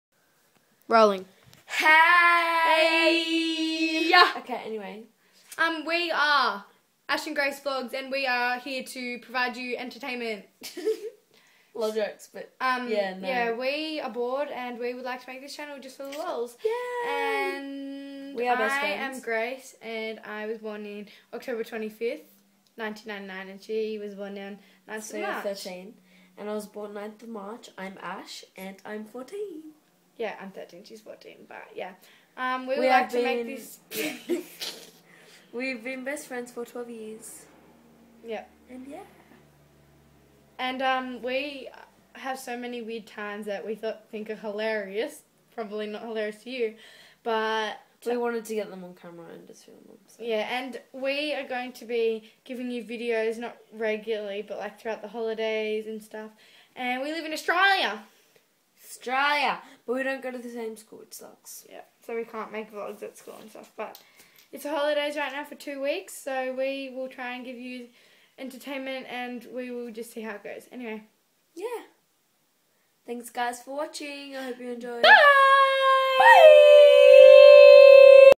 Hi.
Heeeeeeeeeeeeeeeeey

Its ash & grace vlogs. Its our first videos we are doing this because we are 2 teenage girls from australia with boring lives HAHAH and we decided to vlog it laawl !

Hope everyone enjoys our vid vids

Ash Twitter - @BeautyByAshx

(Grace doesnt have a channel or twitter:( )

AHAHHA Laaammmeeeeeee ^ wow im lame cause i said lame.

ex ohh

BYYYEEEEEEEEEEEEEEEEEEEEEEEEE ttyl xo